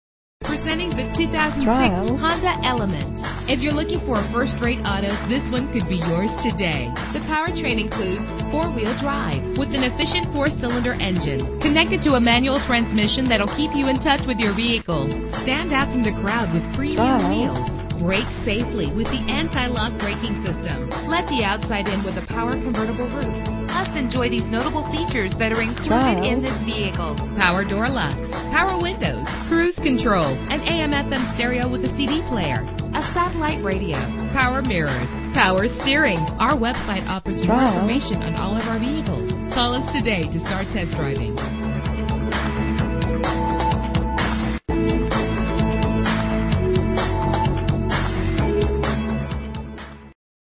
2006 Honda Element - Huntington NY
This vehicle represents similar vehicles available at Huntington Honda.
Contact Huntington Honda at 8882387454, for availability of specific vehicles.
Year: 2006
Make: Honda
Model: Element
Trim: EX-P
Engine: 2.4 liter inline 4 cylinder 16 valve MPFI DOHC
Transmission: 5-Speed Manual
Color: Atomic Blue Metallic
Mileage: 38000
Address:
1055 E. Jericho Tpke